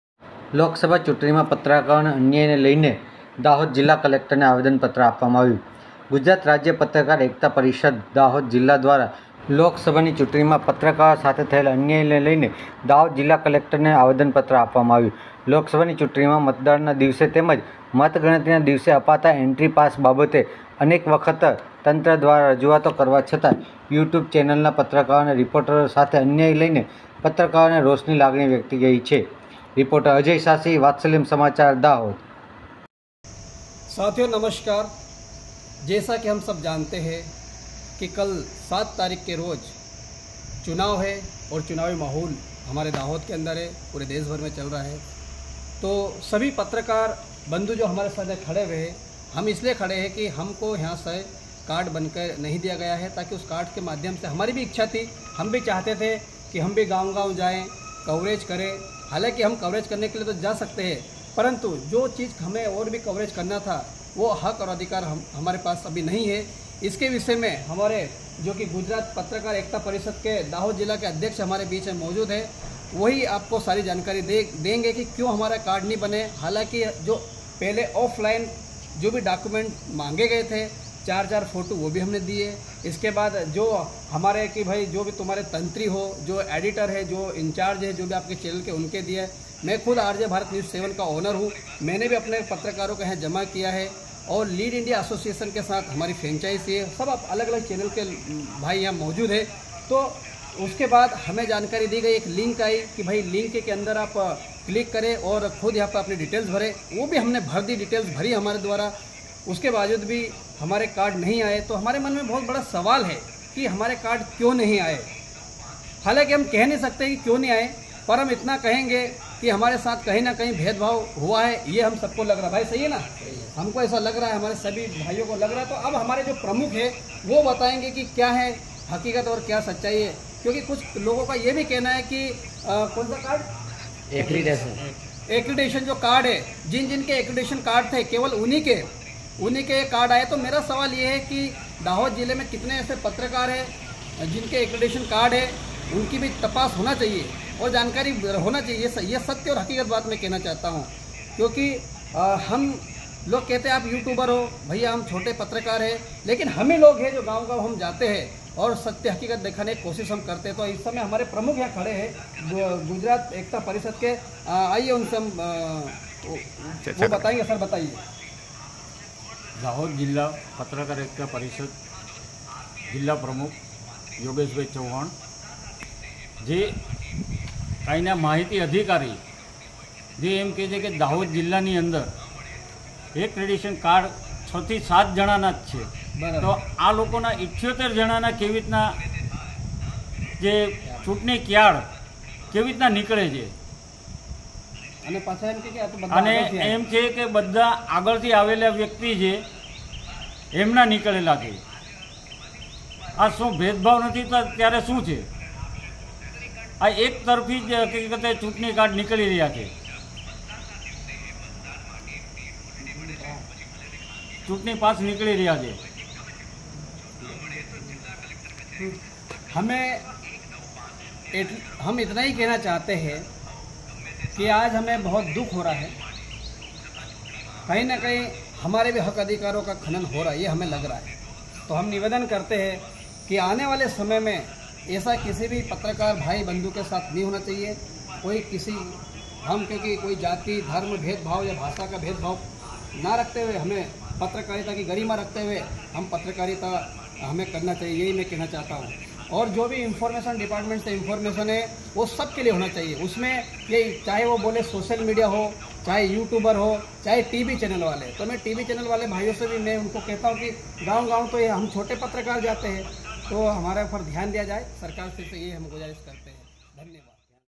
લોકસભાની ચૂંટણીમાં પત્રકારોને થયેલ અન્યાય લઈને દાહોદ જિલ્લા કલેકટરને આવેદનપત્ર અપાયું
દાહોદ જિલ્લા દ્વારા લોકસભાની ચૂંટણીમાં પત્રકારોને અન્યાય લઈને જિલ્લા કલેકટરને આજરોજ આવેદન આપ્યું

*રિપોર્ટર. અજય. સાંસી*